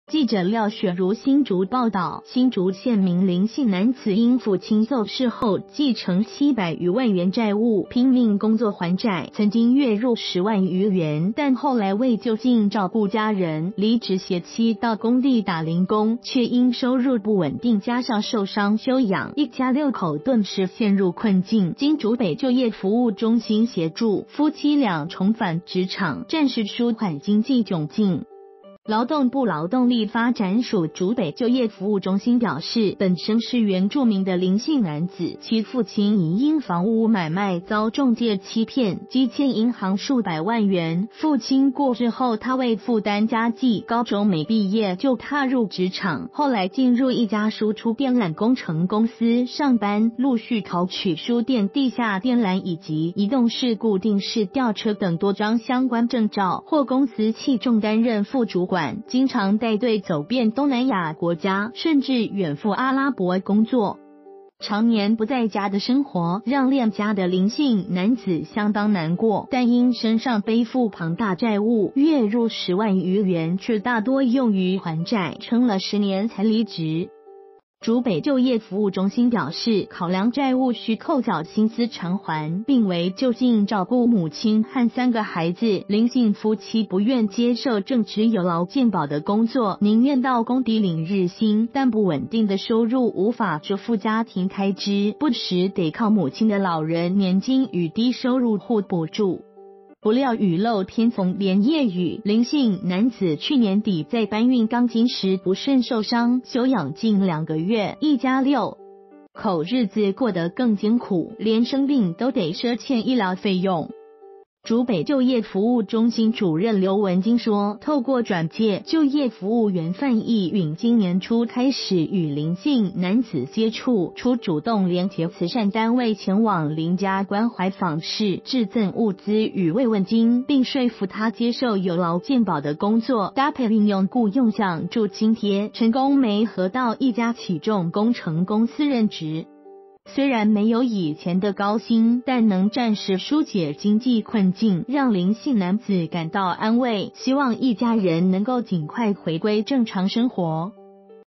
男子工作受傷一家6口陷困境 就服中心拉一把
〔記者廖雪茹／新竹報導〕新竹縣民林姓男子因父親驟逝後繼承700餘萬元債務，拚命工作還債，曾經月入10萬餘元，但後來為就近照顧家人，離職偕妻到工地打臨工，卻因收入不穩定加上受傷休養，一家6口頓時陷入困境；經竹北就業服務中心協助，夫妻倆重返職場，暫時舒緩經濟窘境。

發表評論評論如下。